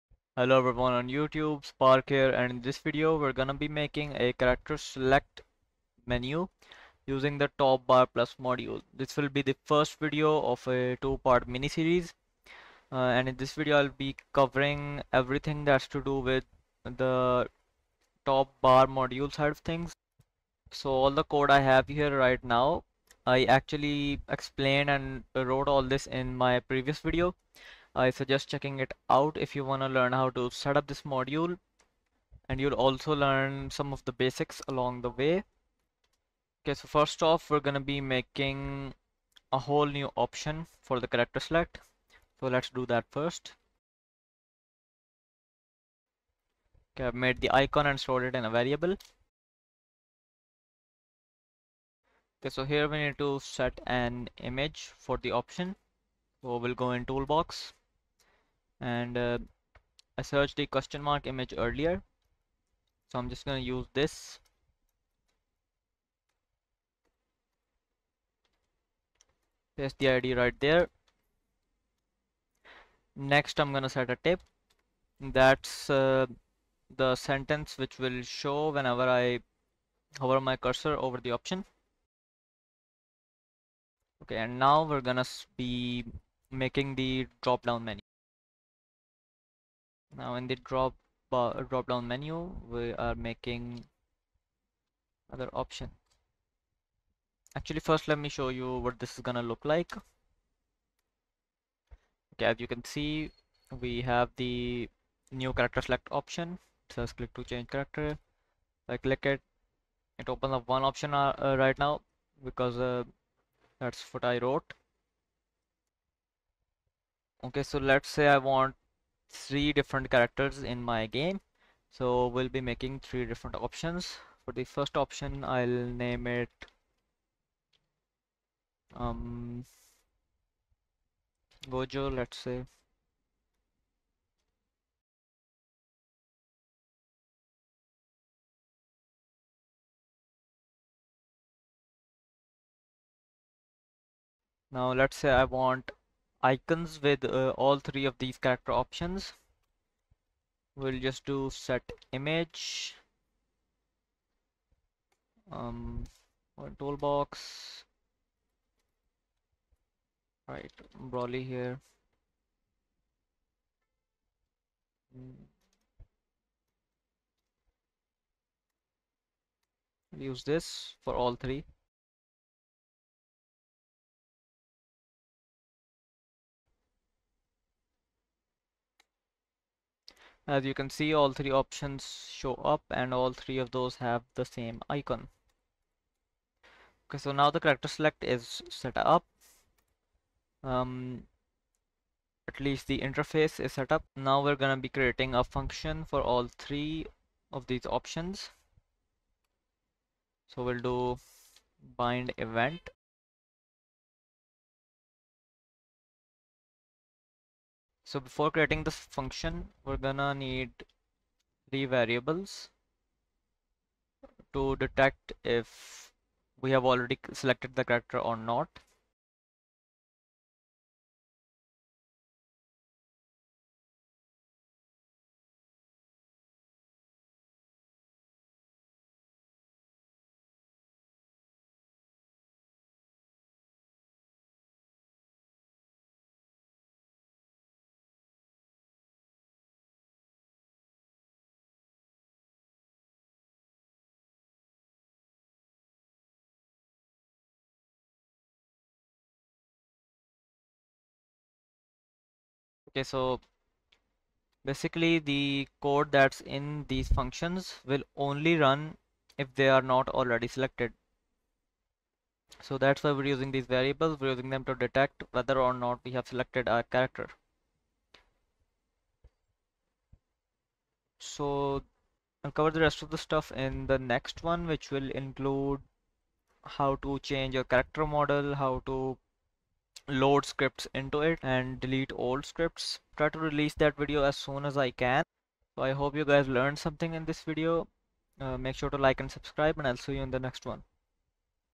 Create an Epic Character Select Menu with TopBarPlus Part 1 | Roblox Studio
In this one, we'll cover the interface side of things. Easy to understand, check out my previous video for the set up.